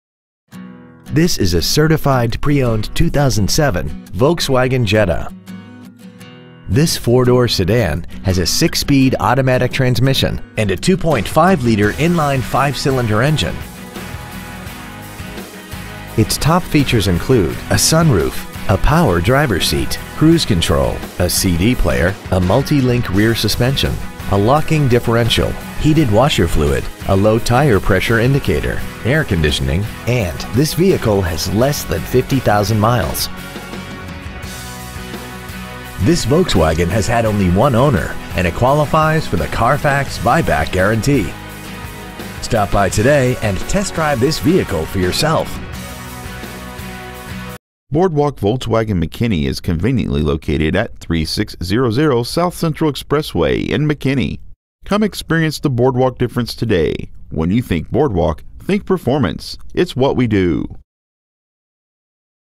2007 Volkswagen Jetta Sedan Certified McKinney TX 75070
Year: 2007
 Make: Volkswagen
 Model: Jetta Sedan
 Engine: Gas I5 2.5L/151
 Trans.: Automatic
 Exterior: Reflex Silver With Anthracite Leatherette
 Miles: 49,546
 Interior: Anthracite V-Tex Leatherette
 Stock: M11J090A

 Used 2007 Volkswagen Jetta Sedan McKinney TX